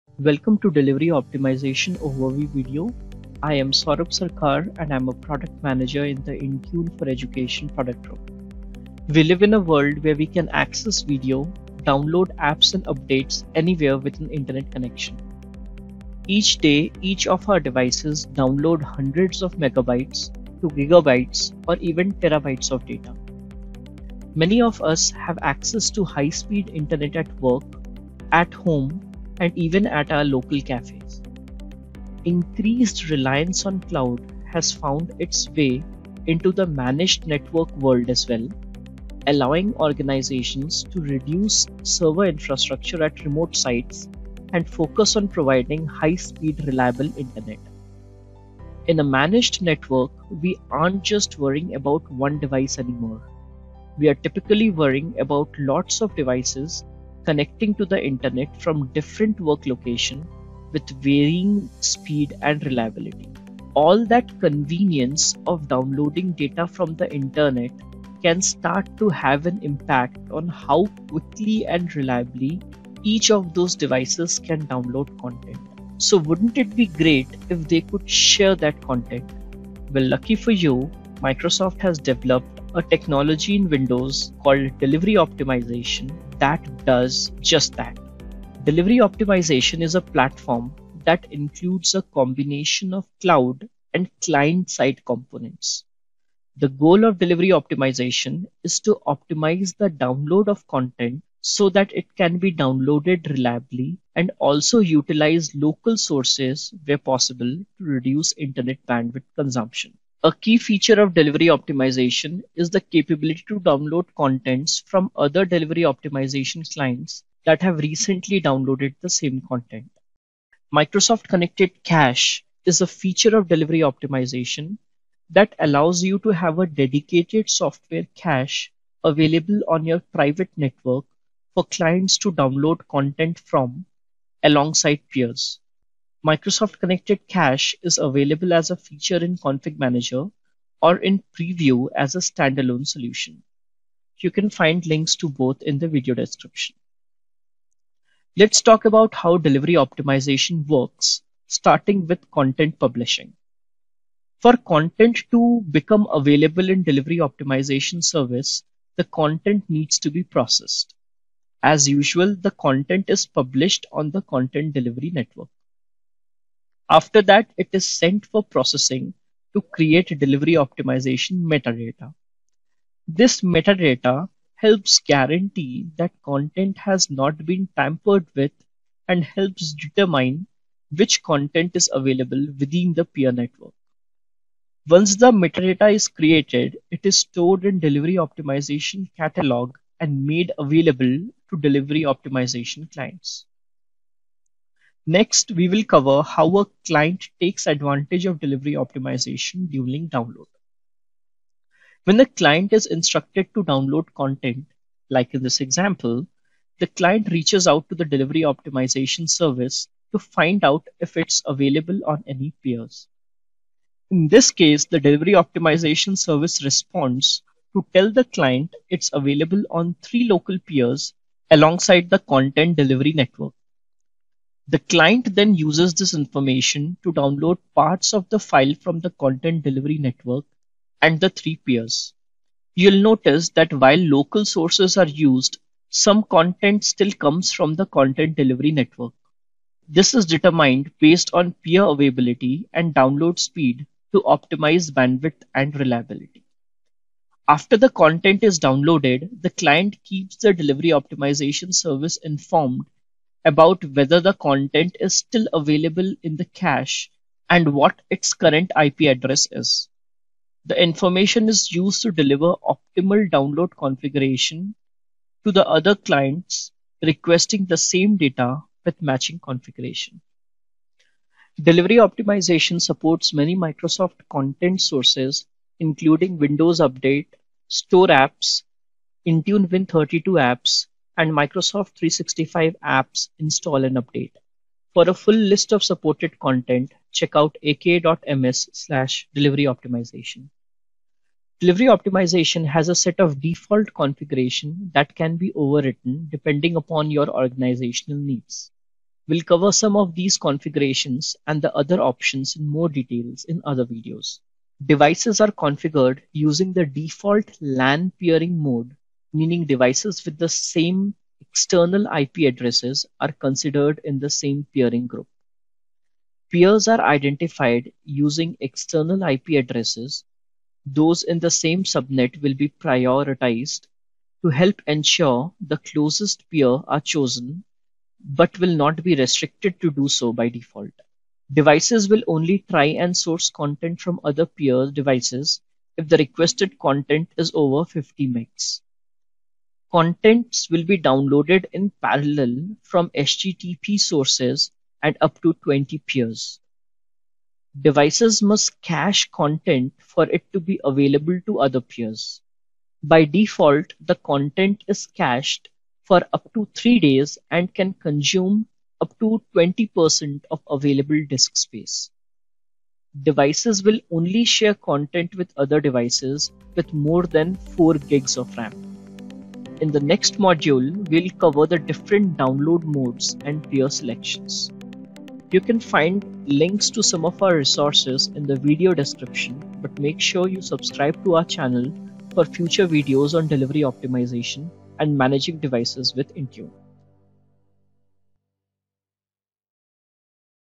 Saving Bandwidth with Delivery Optimization: An Introduction
What is Delivery Optimization in Windows and why is it important? Find out in this video.

Delivery Optimization allows Windows to reliability and efficiently download cloud content.

🕘 Chapters
00:00 Opening
01:33 What is Delivery Optimization?
02:38 How it works
04:58 Default configuration
06:23 Closing